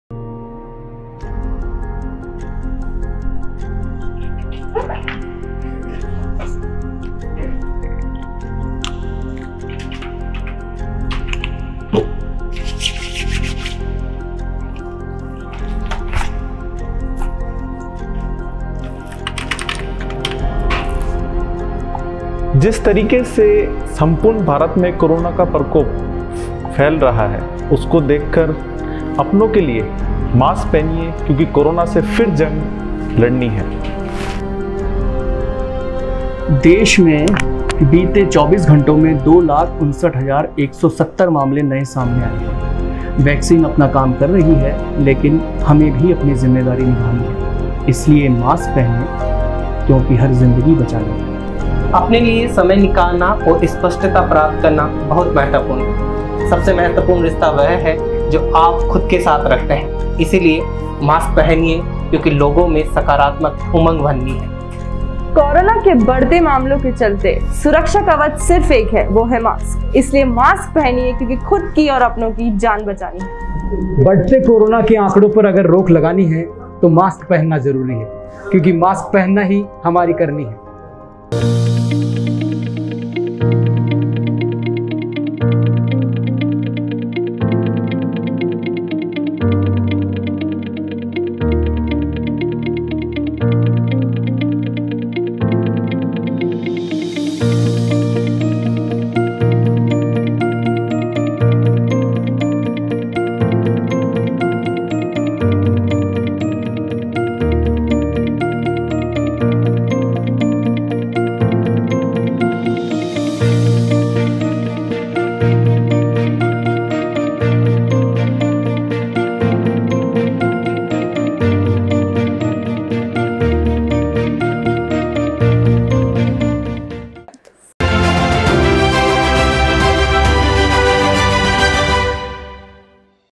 Precautions For Coronavirus |Stay at Home| Stay Safe |Prevention Only Way to Fight Covid 19 Spread.
India is battling a record-breaking rise in COVID-19 infections that has overwhelmed hospitals and led to severe bed and oxygen shortages. Since Independence advises you to stay safe by taking some simple precautions, such as physical distancing, wearing a mask, keeping rooms well ventilated, avoiding crowds, cleaning your hands, and coughing into a bent elbow or tissue. Check local advice where you live and work.